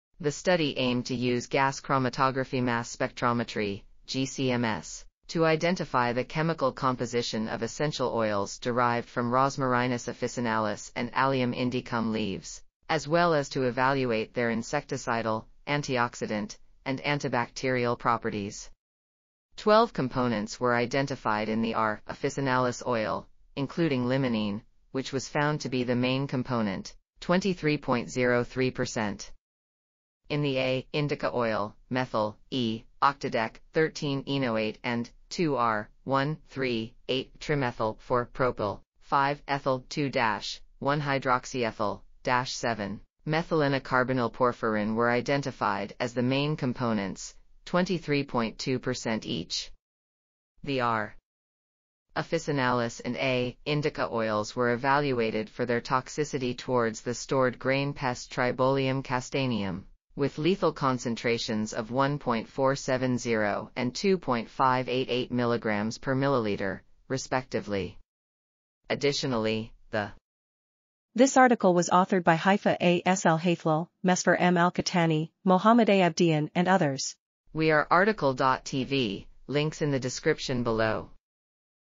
Rosemary and neem methanolic extract: antioxidant, cytotoxic, and larvicidal activiti... | RTCL.TV
### Keywords ###

### Article Attribution ###
Title: Rosemary and neem methanolic extract: antioxidant, cytotoxic, and larvicidal activities supported by chemical composition and molecular docking simulations
Authors: Haifa A. S. Alhaithloul, Mesfer M. Alqahtani, Mohamed A. Abdein, Mohamed A. I. Ahmed, Abd El-Latif Hesham, Mohammad M. E. Aljameeli, Reem N. Al Mozini, Fatehia N. Gharsan, Suzan M. Hussien ,and Yasser A. El-Amier

### Video Timestamps ###
0:00:00 - Summary
0:01:10 - Title
0:01:17 - Outro
0:01:21 - End